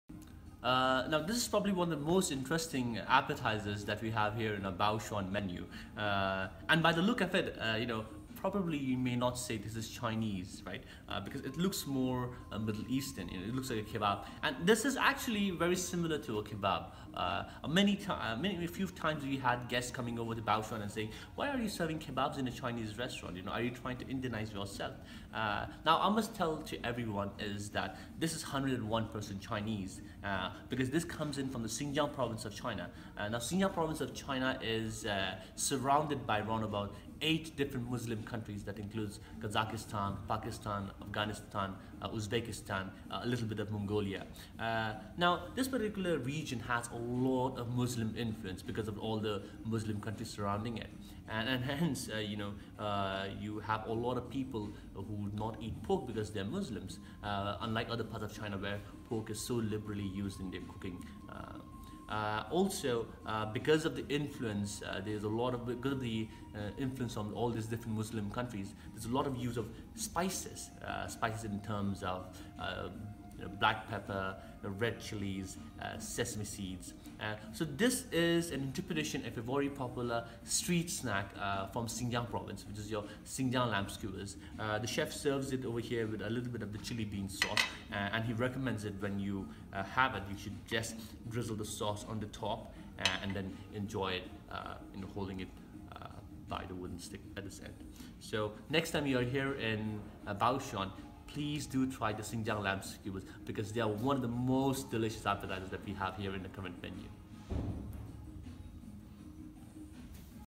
Sichuan Lamb Skewers at Baoshuan
Sichuan Lamb Skewers, arguably one of the most interesting appetizers on the menu at Baoshuan are mistaken to have Middle Eastern origins because of their resemblance to kebabs. Watch the video to know why this appetizer is actually acompletely authentic Chinese dish.